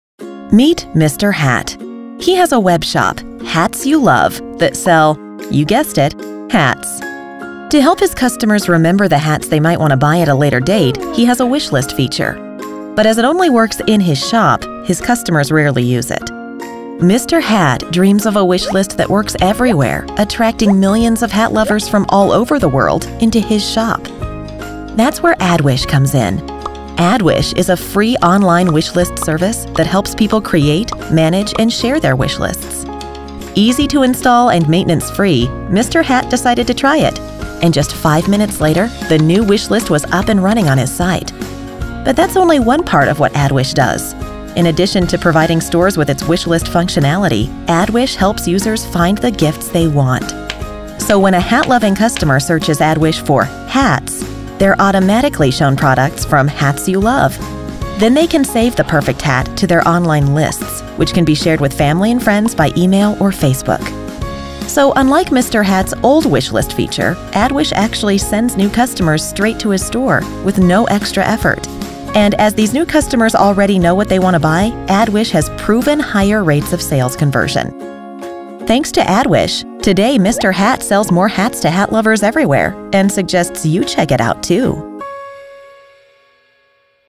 addwish for Businesses
Add addwish. Add sales

addwish is the easiest way to get a tried and tested wish list on your web shop. Completely free, easy and secure to install and maintenance-free, a wish list lets your users share the products they want with people who want to buy them.